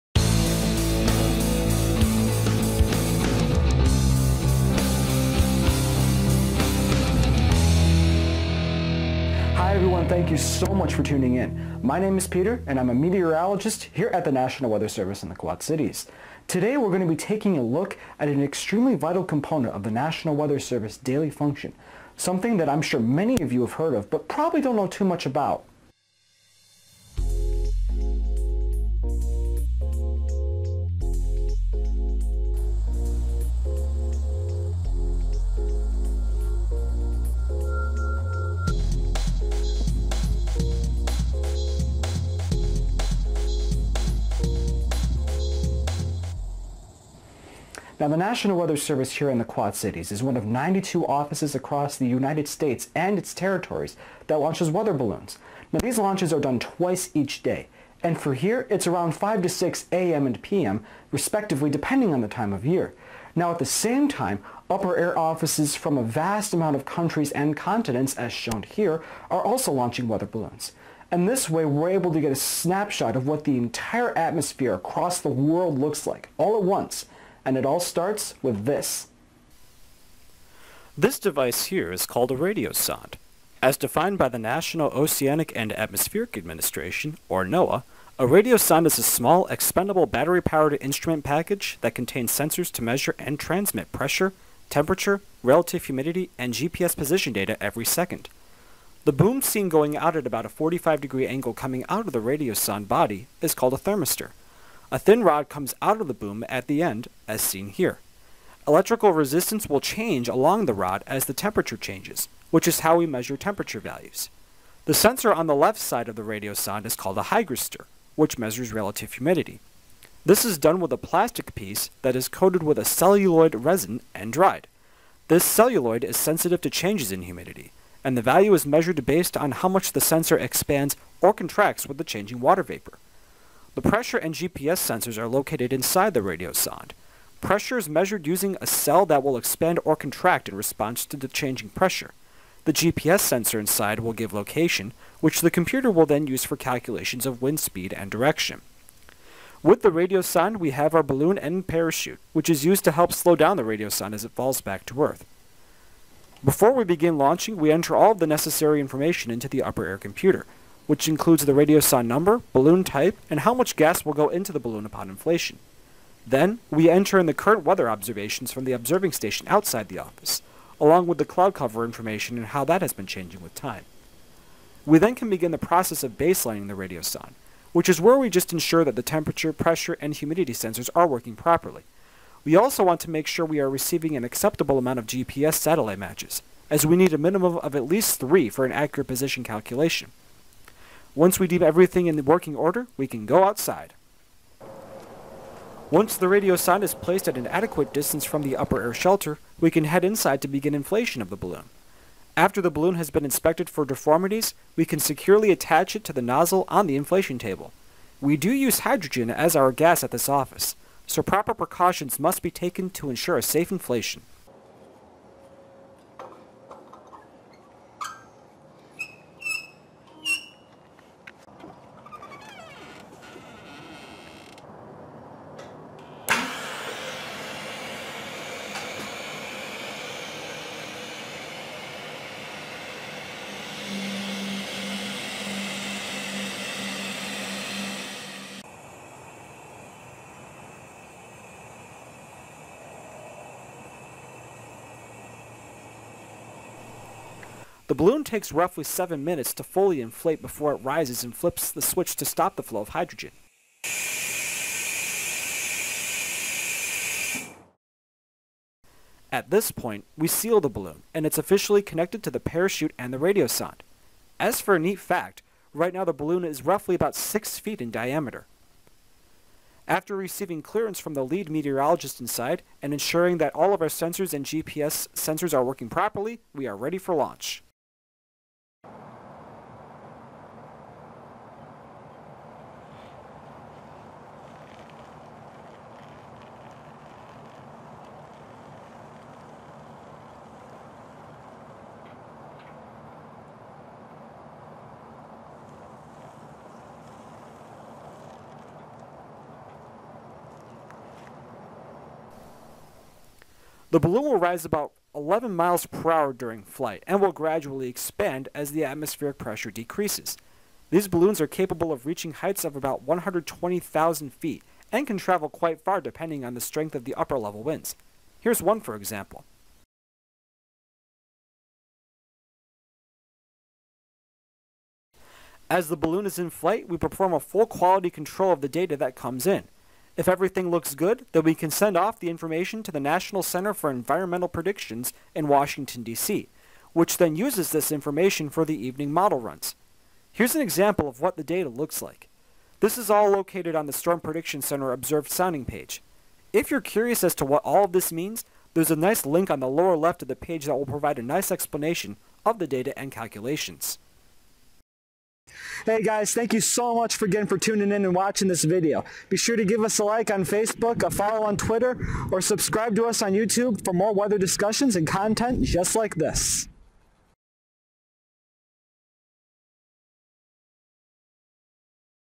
Upper Air (Weather Balloon) Program
Upper air observations began in the mid 1700s in eastern Europe, and have evolved over the past few decades to a point where they play a vital role in weather forecasting. Most of you have probably heard of the weather balloon from various sources, but do you know what they measure, and the preparations behind it? The NWS Quad Cities IA/IL explains!

Quad Cities, IA/IL